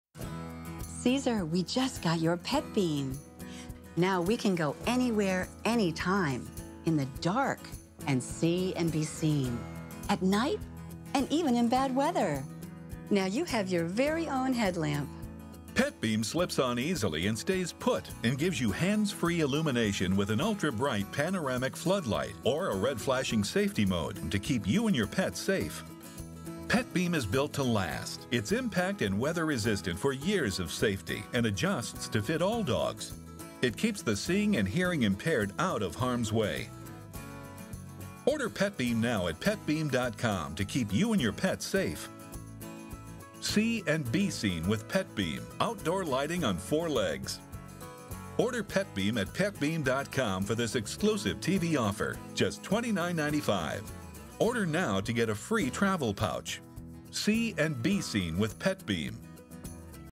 Pet Beam The World's Best Pet Collar as seen on television commercials
Petbeam, keep yourself and your canine companion safe in any low light environment.

PETBEAM®  gives you and your pet light where and when you need it, even if it’s dawn, dusk, pitch-black outside or bad weather like fog, rain, snow, or sleet is reducing visibility.

See and be seen, hands-free, with PETBEAM: Outdoor Lighting On Four Legs, the first of its kind, non-pendant style, collar mounted safety light designed and engineered in the U.S. for your best friend (BFF).

Whether you want to walk, hike, camp, jog, skip, let Rover out to romp, get out of Dodge (GOOD), live on a busy street or just feel vulnerable, PETBEAM keeps you and yours highly visible anywhere, anytime, on or off leash.